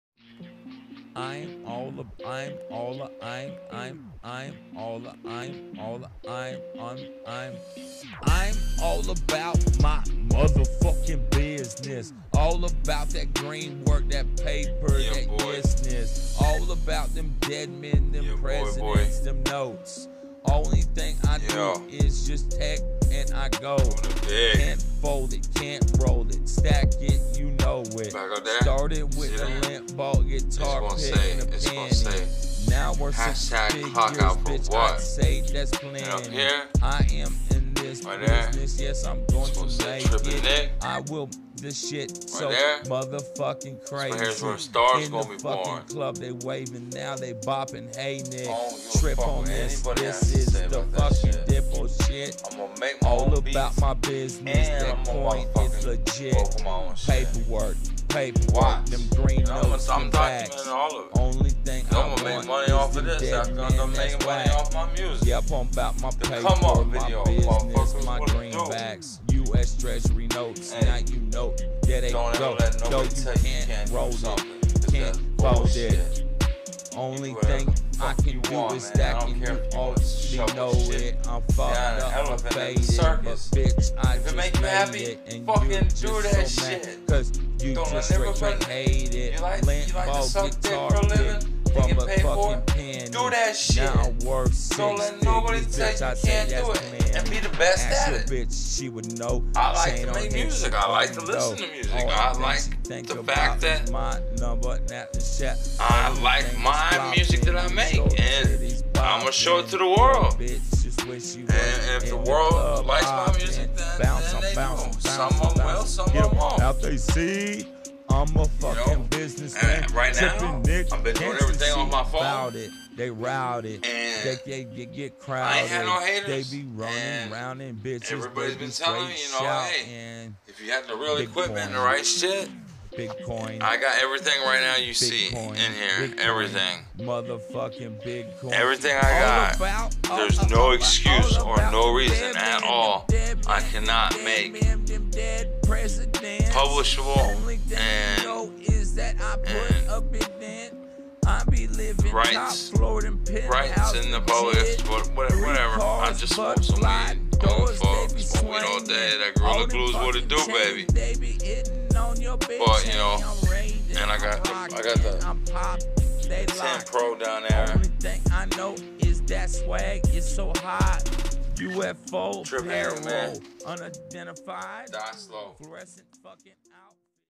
Documentary the, come up
This is a YouTube blog / documentary about a star being born chasing his dreams and going to make it to come true and I'm documenting it.

           Sincerely, TrippinNick

  Bandlab.com/trippin_nick.      battlemeapp/@.trippinnick